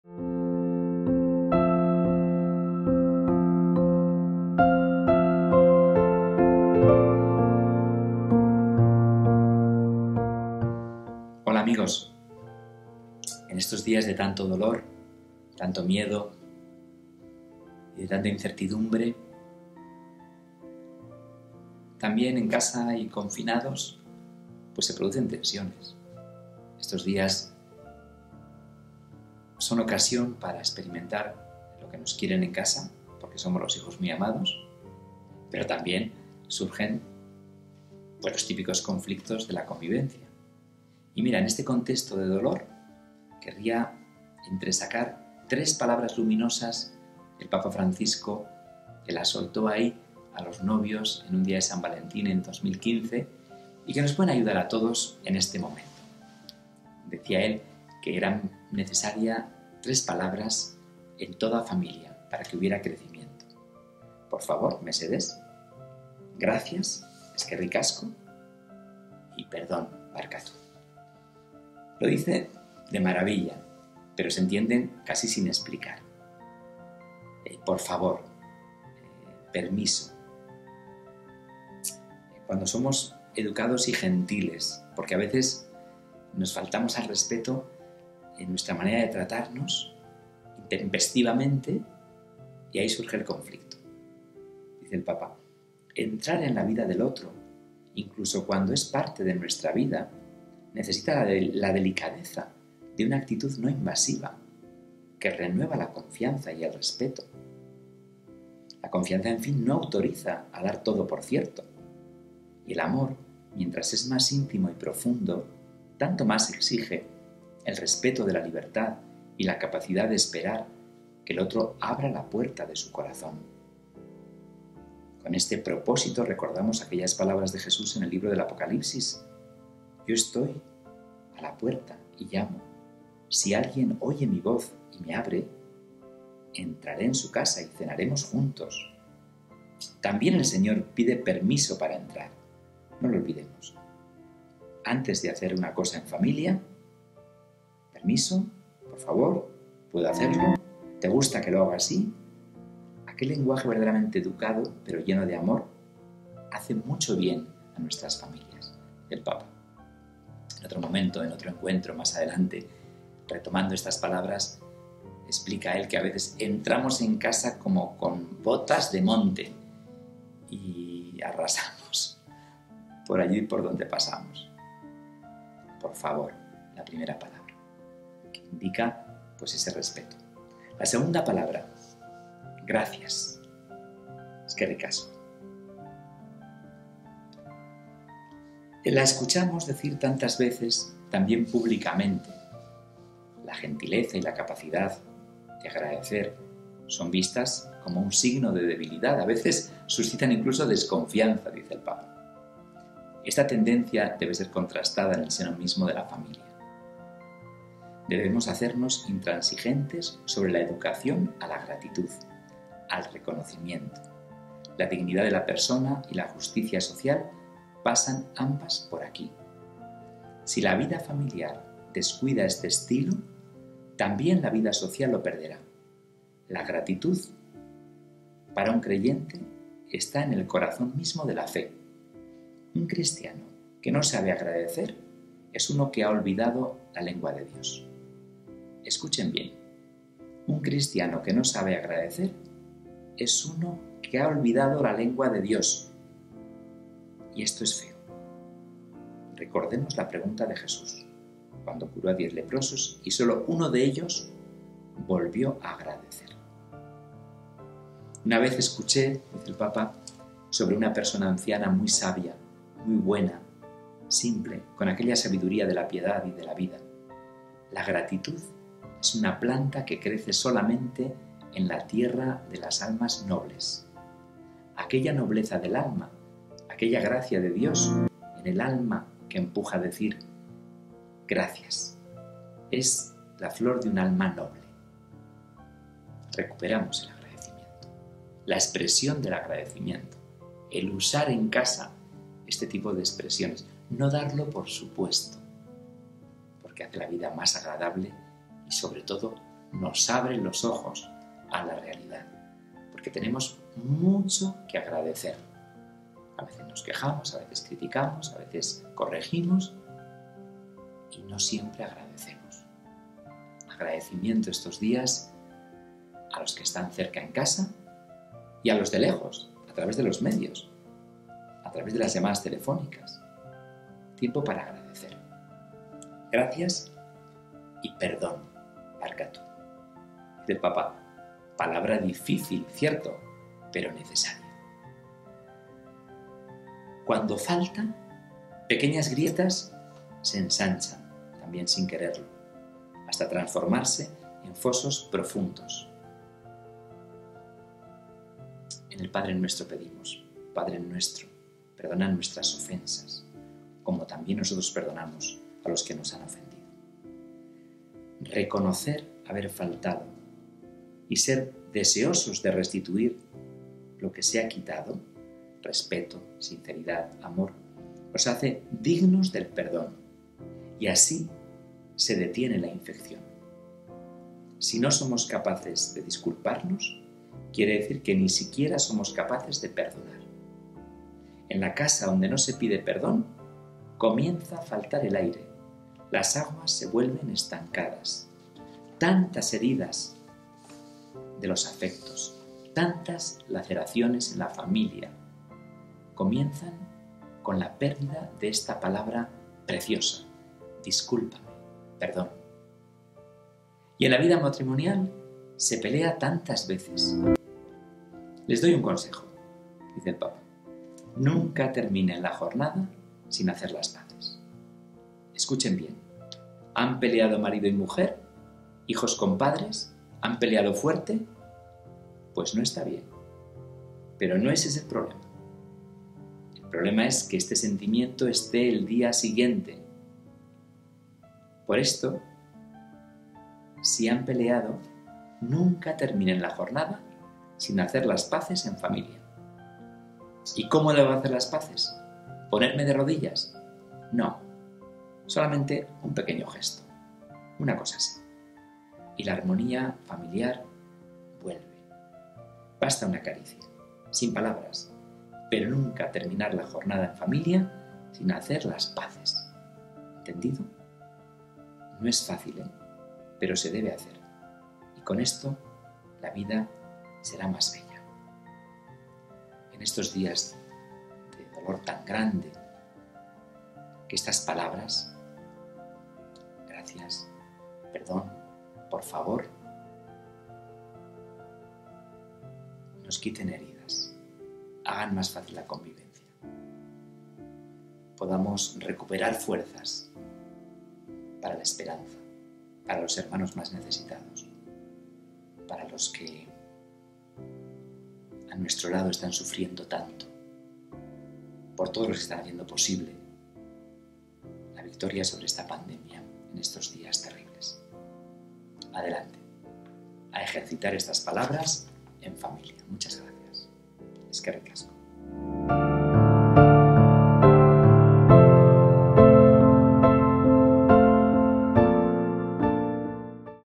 Por favor, gracias y perdón | Palabras de nuestro Pastor
El Obispo de Vitoria, D. Juan Carlos Elizalde, se sentará con nosotros de lunes a viernes para compartir una reflexión cuaresmal ante la situación generada por el virus COVID-19 y caminar así juntos hacia el Misterio Pascual.